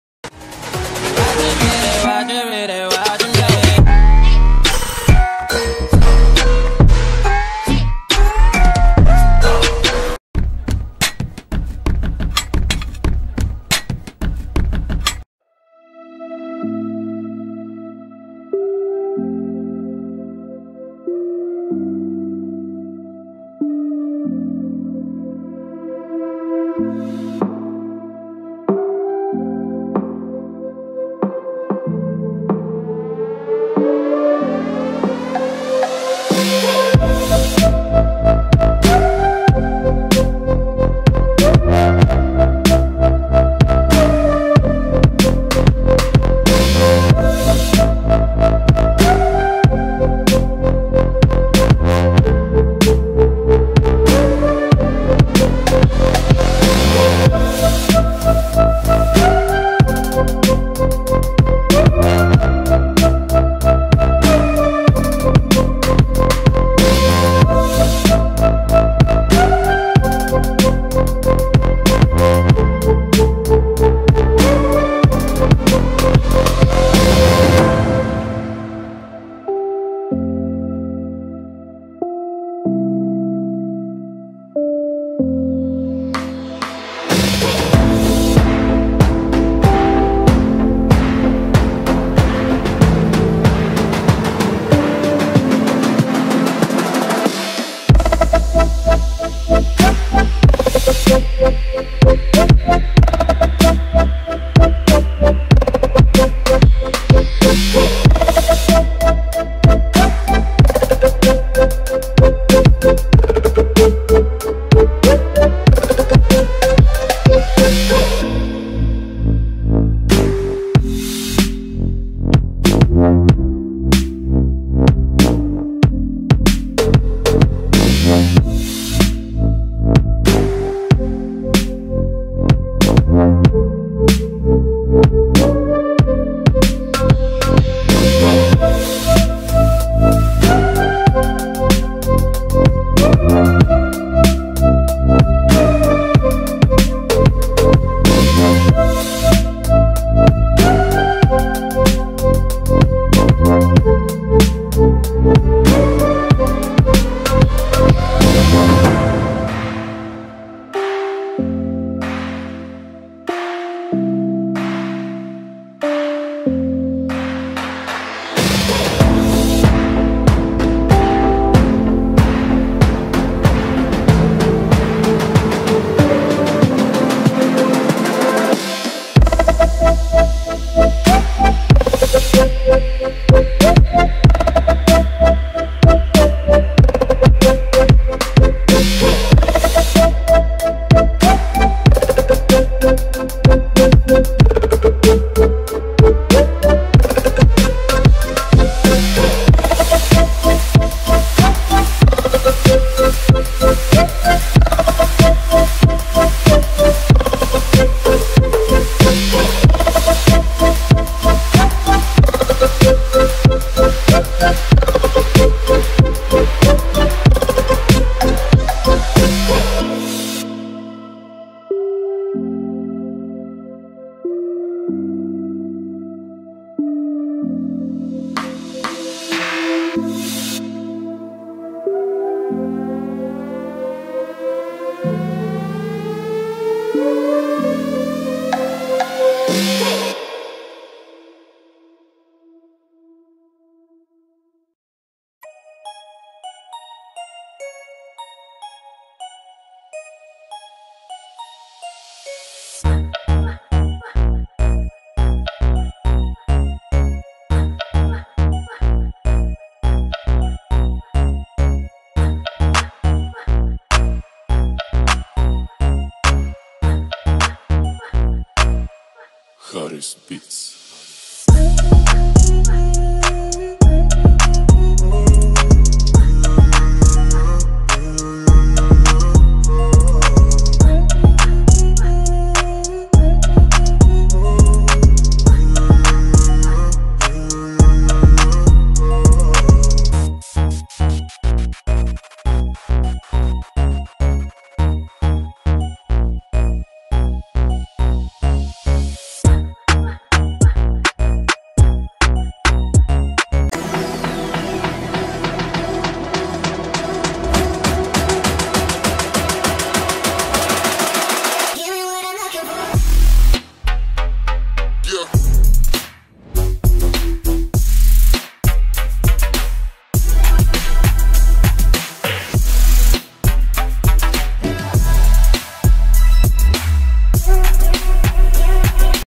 Bollywood Actor Sonu Sood Quiz Challenge 2021
Hi and Welcome to QUIZ CHARM
Sonu Sood  Quiz Challenge Video Game with answers

Video by QUIZ CHARM